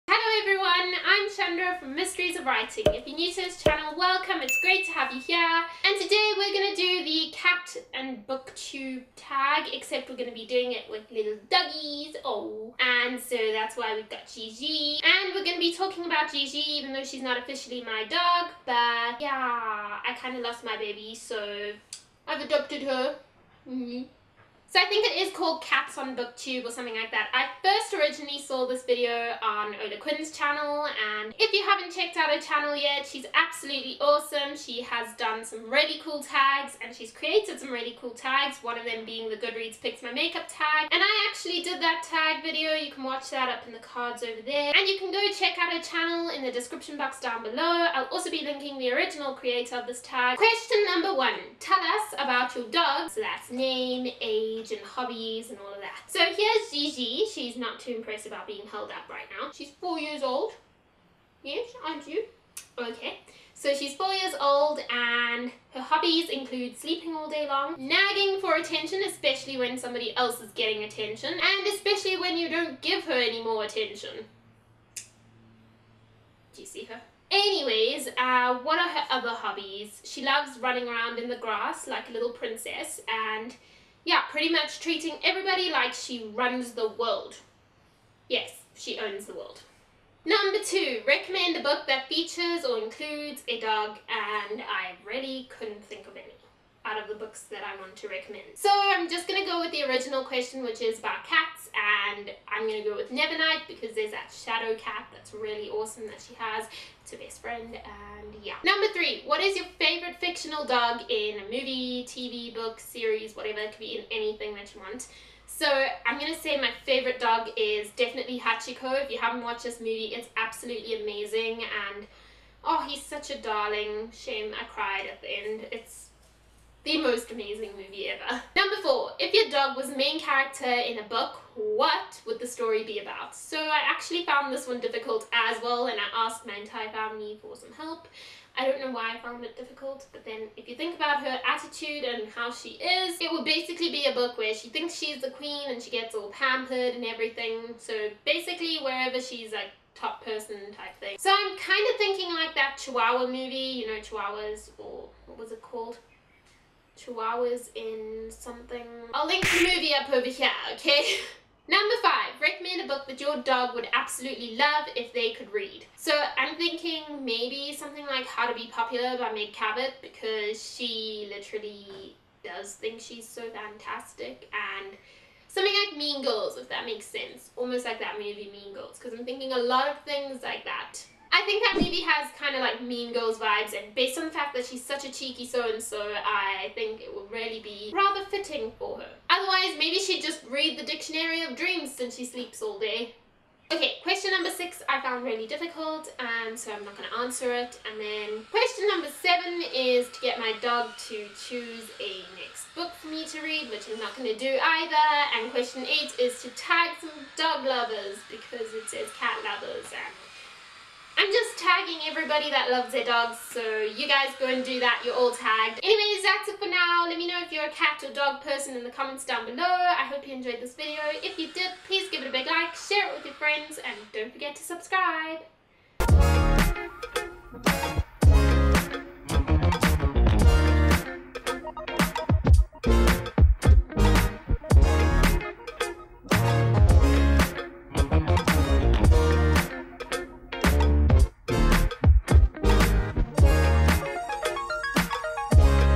DOGS AND BOOKS TAG ft. GIGI ! 🐶🐾 📚 | by Mysteries of Writing
Gigi joined us for a tag video today that's all about dogs and books! This tag is similar to the well known cats and books tag except the spotlight is on dogs! I hope you all enjoy the video and don't forget to do it as well if you LOVE dogs! 💖🐶

QUESTIONS:

1. Tell us about your dog(s)! Name, age, hobbies?
2. Recommend a book that features, includes or mentions a dog?
3. What’s your favourite fictional dog? This can be from literature, tv, movies, games etc
4. If your dog was the main character in a book, what sort of book would it be?
5. Recommend a book that your dog would enjoy if they could read.
6. Which book or story would benefit from the inclusion of one or more dogs?
7. Have your dog pick your next read! (optional)
8. Tag some dog lovers!

If you want to find rare, unique and out of print books